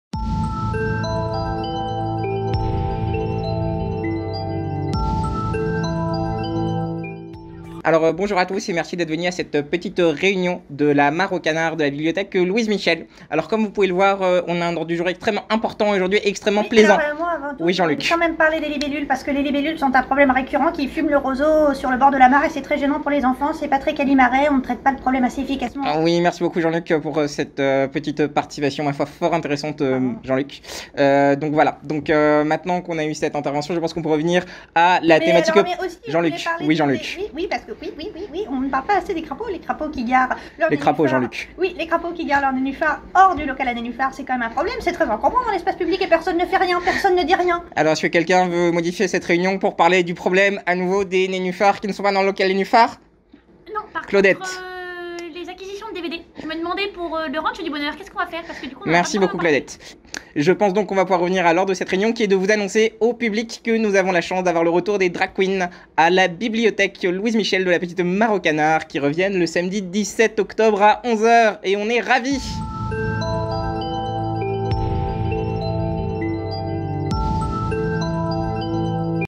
Louise & Michel - Octobre 2020
En octobre, nous retrouverons les Drag Queens à la bibliothèque ! Ce sera le 17 octobre, à 11h, pour une nouvelle heure du conte. Ce sera masqué et sur inscription, n'hésitez pas à nous appeler dès maintenant pour réserver ! :)